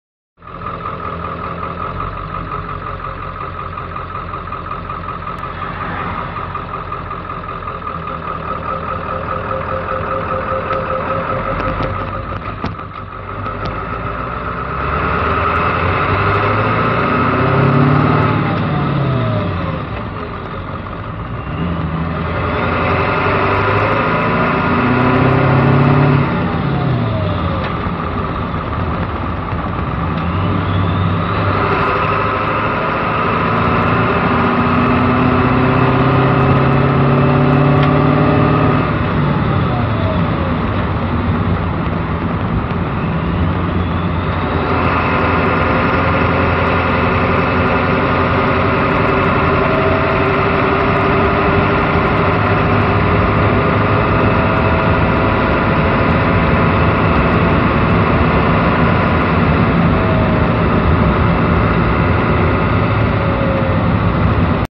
Going through the 5 speeds in the 1994 7.3 with split shot injectors
little shifting in a 7.3 I use to own as a young man. sold about 2 years ago.